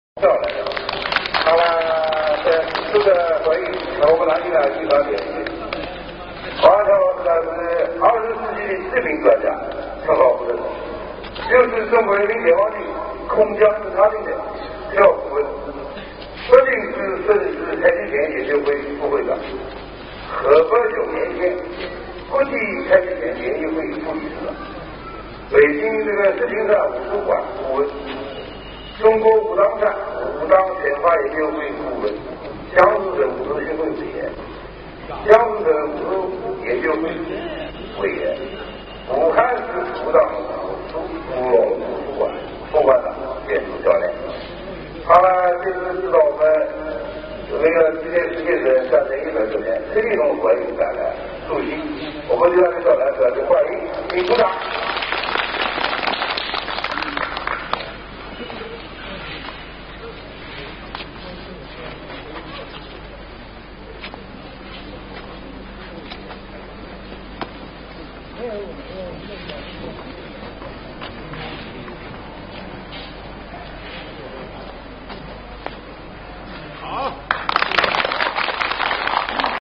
Huang Wan-xiang - Sun style baguazhang
Huang Wan-xiang, teacher from Huaiyin in Jiangsu province, demonstrates Sun style baguazhang which he learnt from Sun's disciple Wan Liang.